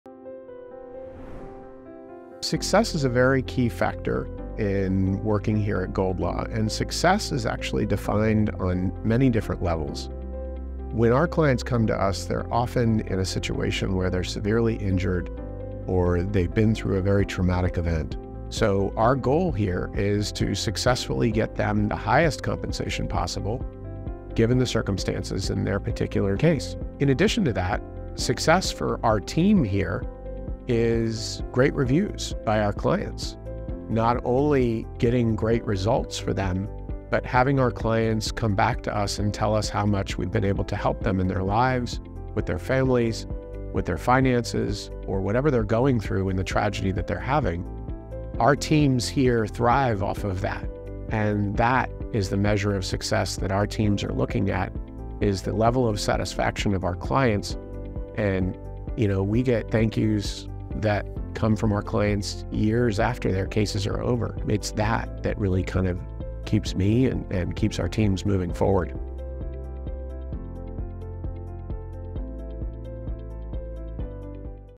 GOLDLAW Core Values: Success
Chief Legal Officer, Spencer Kuvin, discusses GOLDLAW's core value of success.